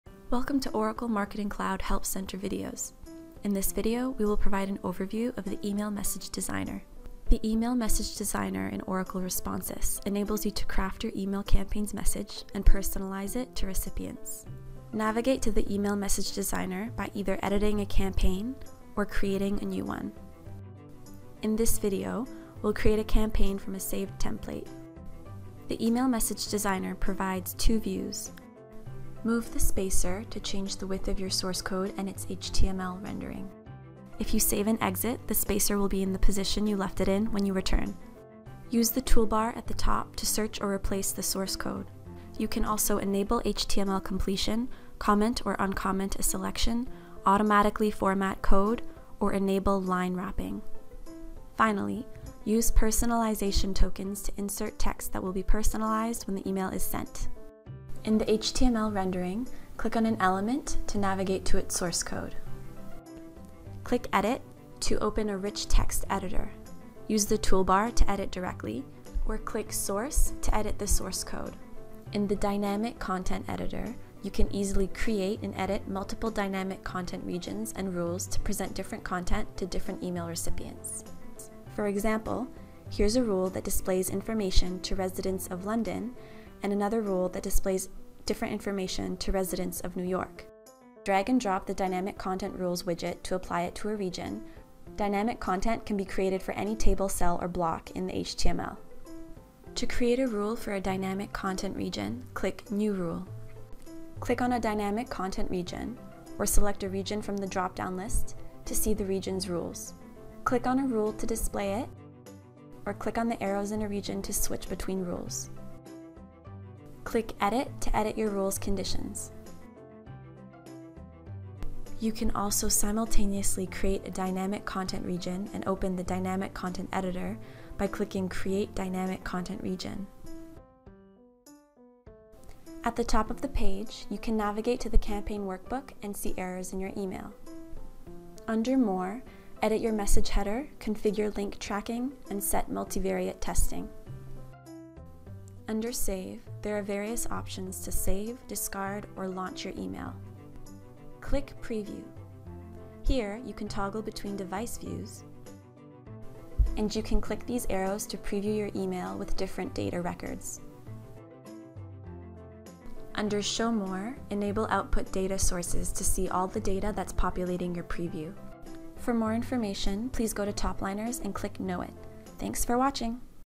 Oracle Responsys - Email Message Designer Overview
This video will give you an overview of the Email Message Designer in Responsys.